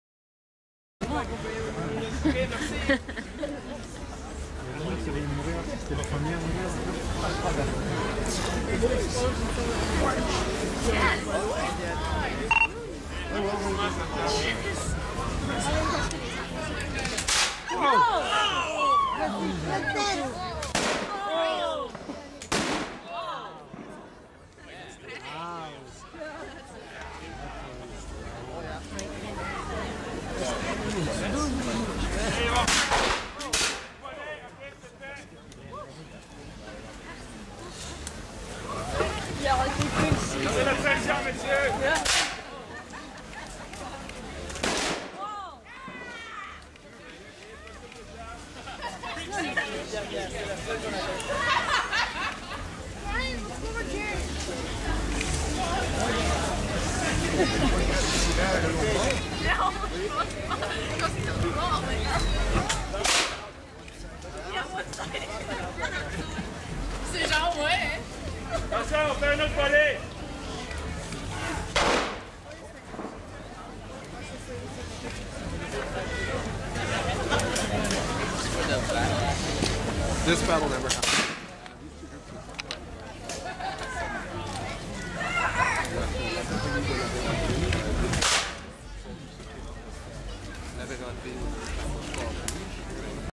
Skirmish 2 part 1
Epic battle between La Compagnie et les Anglois